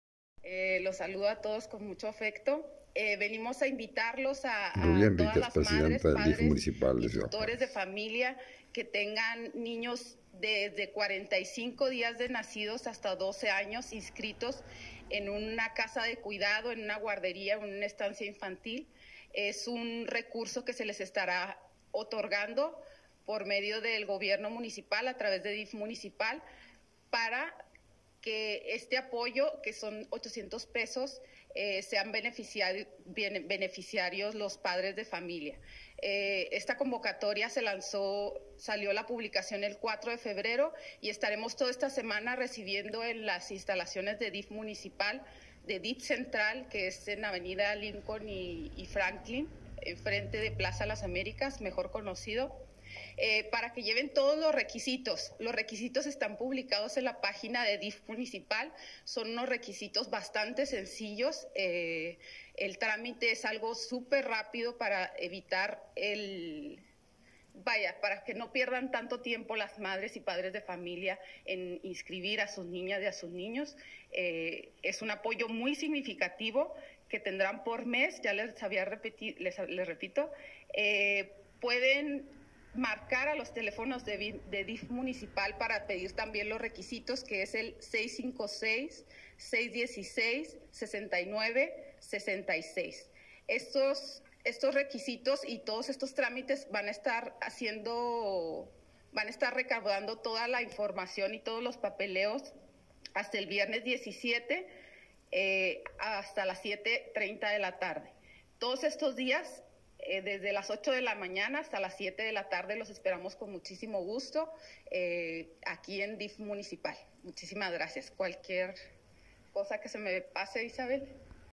El DIF Municipal otorgará apoyos económicos a familia de bebes y niños, anunció Rubí Enríquez
Cd. Juárez. - La Presidenta del Dif municipal, informó que estos apoyos se ampliaron por desicion del Presidente a 24 millones.
Cd Juárez. Dicha funcionaria del Dif Municipal indicó que los apoyos económicos anunciados, se otorgarán cumpliendo con requisitos sencillos, que se indican en la convocatoria, y están encaminados a beneficiar familias que tengan niños registrados en estancias adscritas al Dif municipal de Juárez.

Asimismo señaló que los beneficiados podrán registrar hasta 3 hijos, que cuenten con la edad de 45 dias de nacidos hasta 12 años.
La referida funcionaria comentó que los niños que podrán ser beneficiados, serán 3600 y podrán ser registrados hasta el día 17 de febrero, en las oficinas centrales de dicha institución.